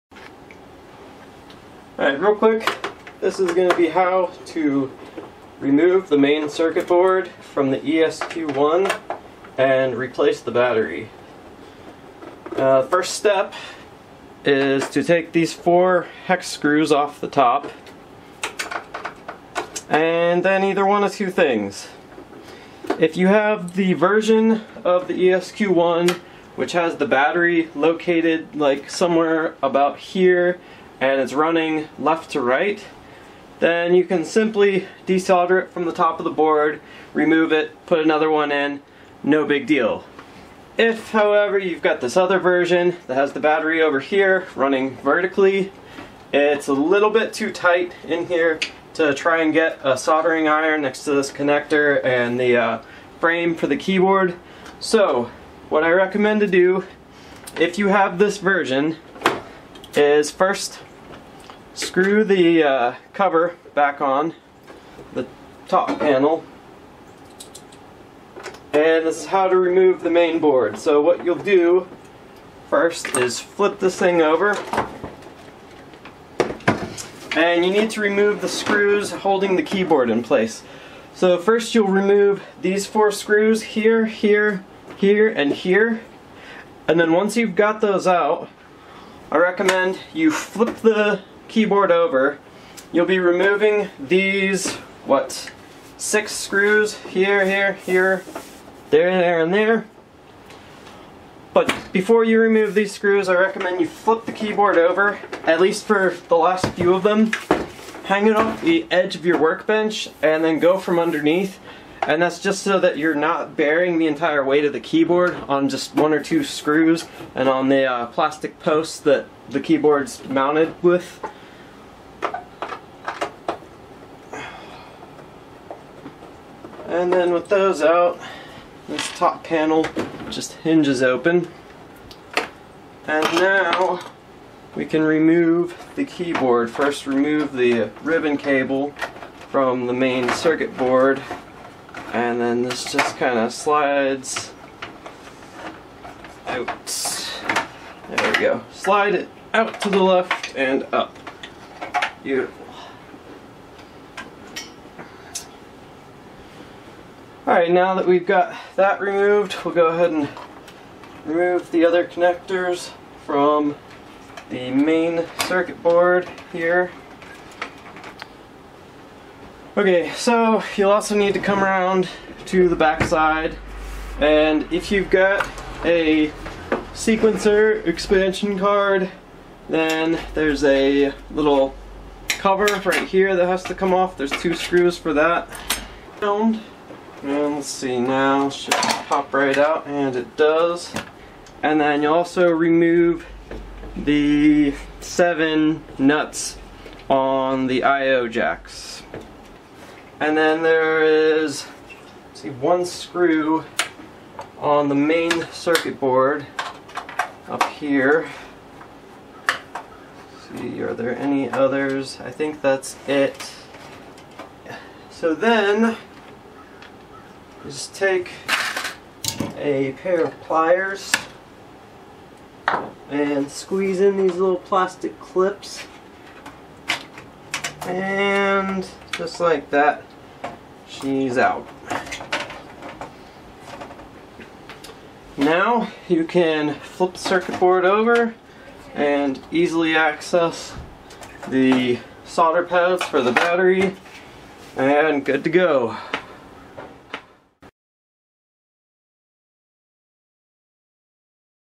Ensoniq ESQ 1 Accessing Internal Battery for Replacement (Soldering Required)
1:18 To skip to what screws to remove to get at the main PCB.